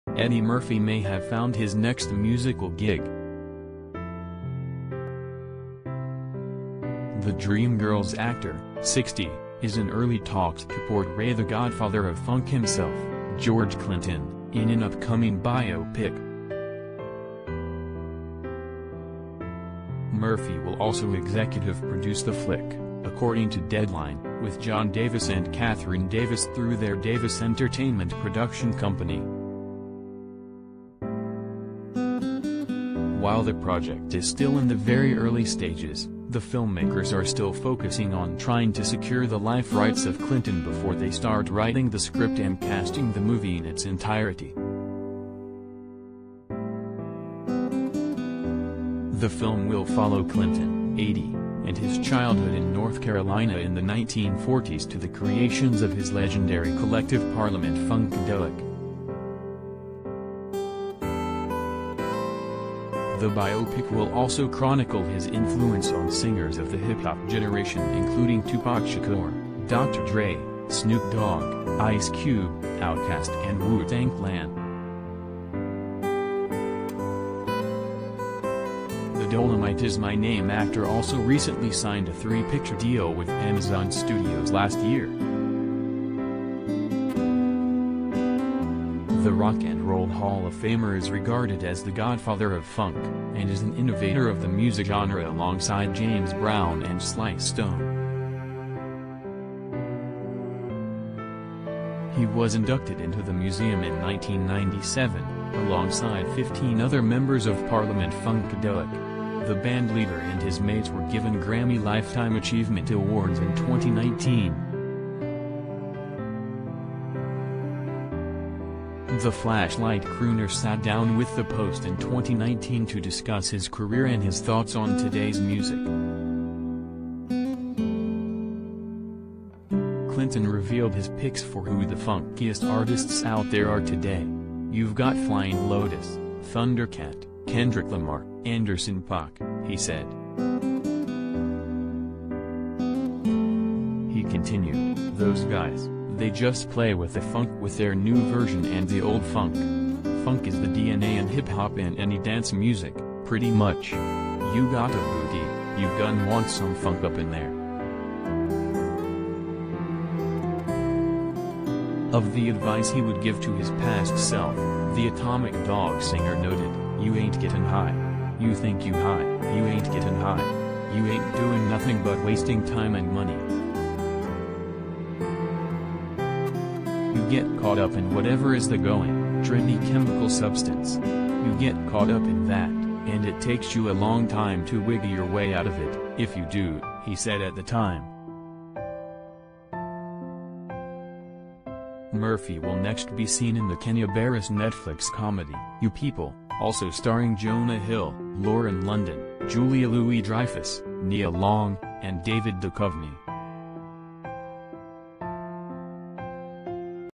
Eddie Murphy in talks for George Clinton ‘Godfather of Funk’ biopic
Eddie Murphy may have found his next musical gig.
The “Dreamgirls” actor, 60, is in early talks to portray the Godfather of Funk himself, George Clinton, in an upcoming biopic.
Murphy will also executive produce the flick, according to Deadline, with John Davis and Catherine Davis through their Davis Entertainment production company.
While the project is still in the very early stages, the filmmakers are still focusing on trying to secure the life rights of Clinton before they start writin